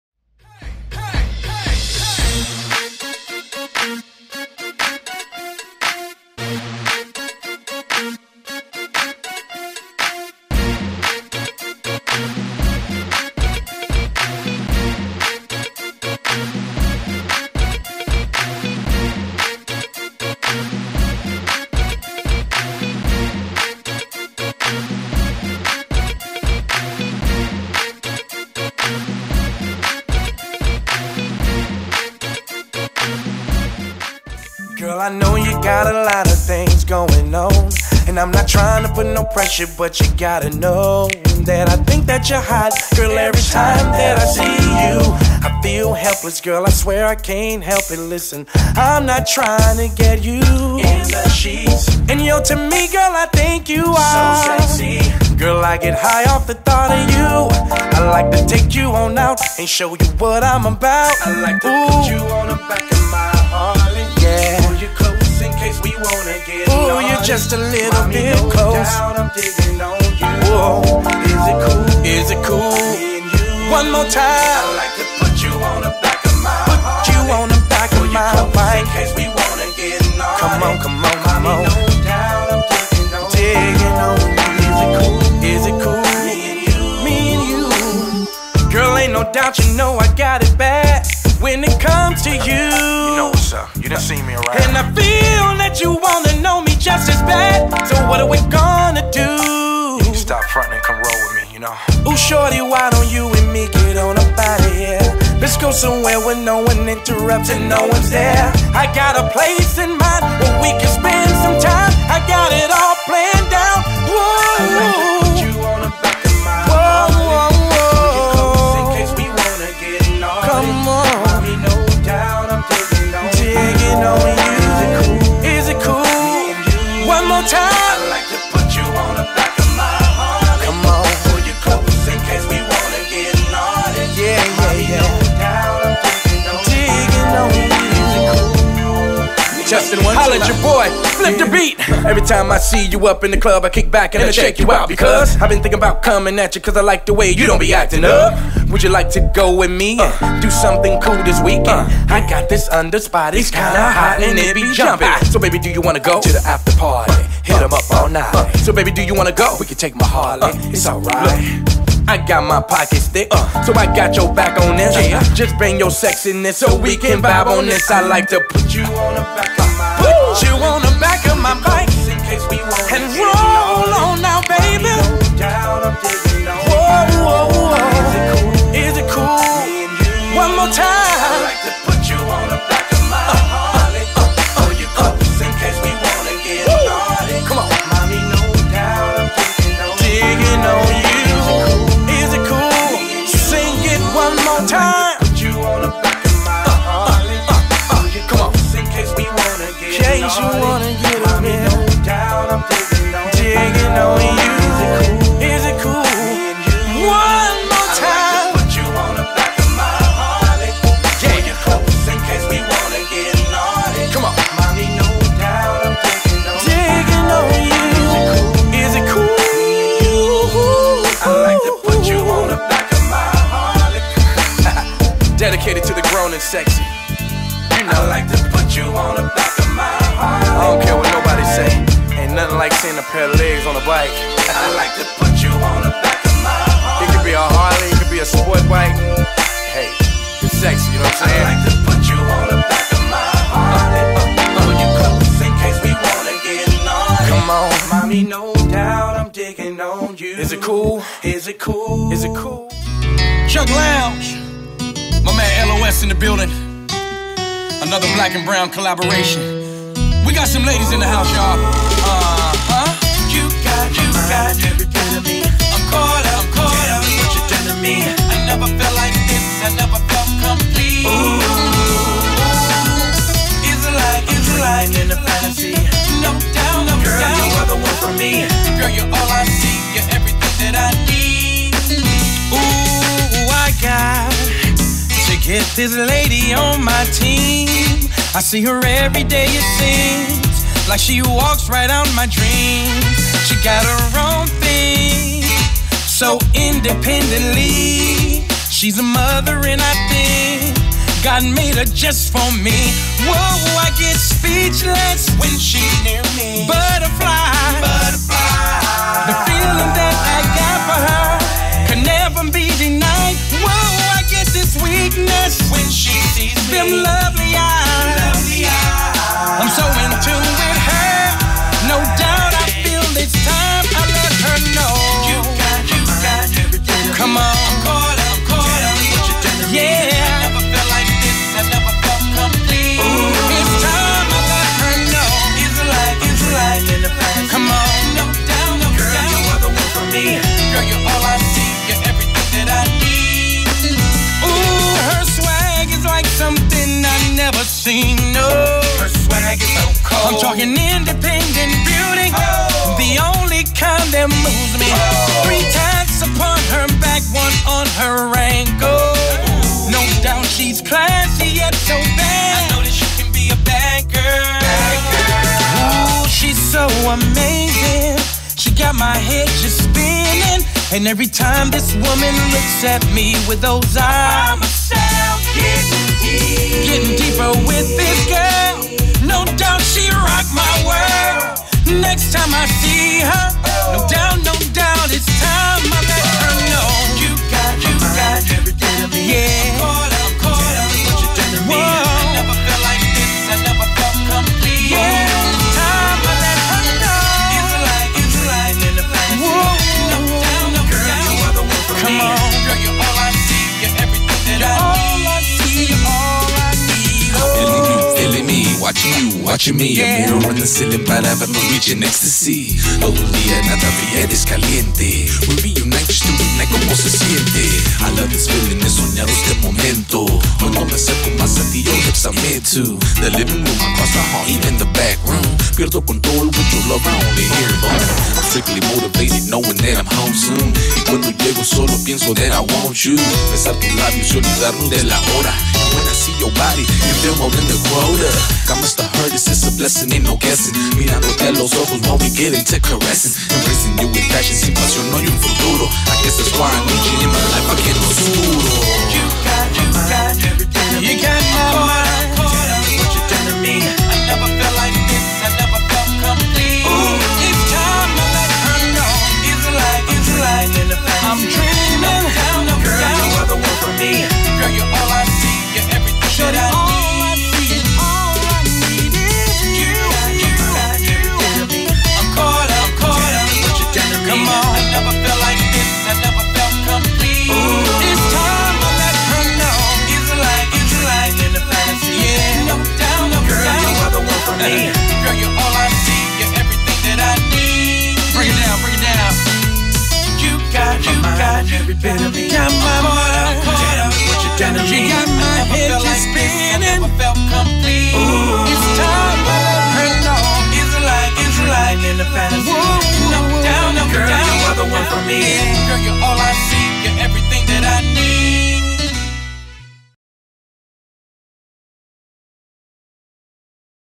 Soulgear Promo 2
Hot August Nights Musical Summer Fashion Show presented by Ronnie McDaniel 2013.  Music by Chuck Lounge